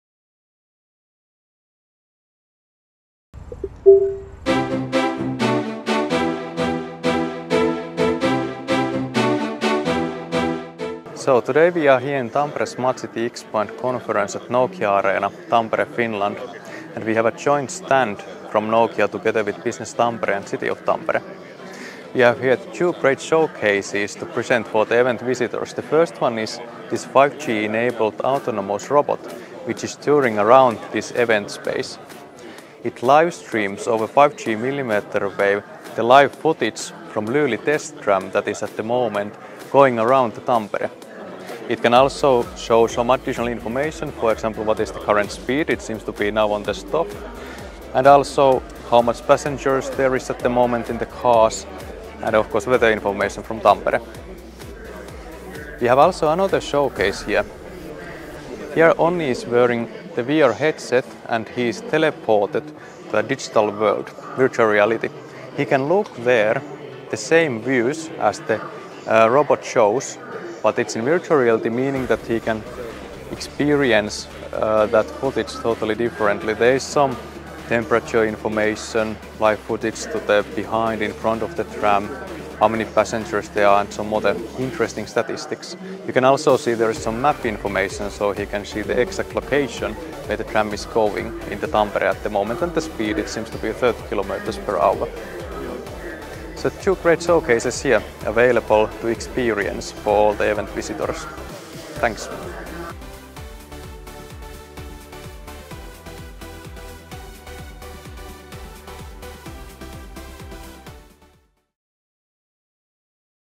Impact of 5G Millimeter Wave Technology on Cutting-Edge Display Robots and Immersive VR Experiences
Nokia's Network as Code team participated in the Tampere Smart City Expo and Conference 2023, hosted at Nokia Arena for innovative showcases and collaborations

One of the highlights of TSCEC this year was the cutting-edge display robot showcase which was made possible by leveraging the advancements of 5G millimeter wave technology. Another showcase was the VR experience, where individuals could immerse themselves in real-time footage from a nearby tram.

The robot showcase was a collaboration between Nokia, the City of Tampere, and Business Tampere. Skoda Transtech provided real-time footage and passenger status graphs, while Kepit Systems delivered the content through their robust content delivery network (CDN) to the robot and VR glasses. The team at Autonomous Units and JTA Connection handled the delivery, programming, and maintenance of the moving robot.

We are always looking to push the boundaries of innovation at Nokia Arena. What other showcases would you like to see in Finland's most dynamic venue?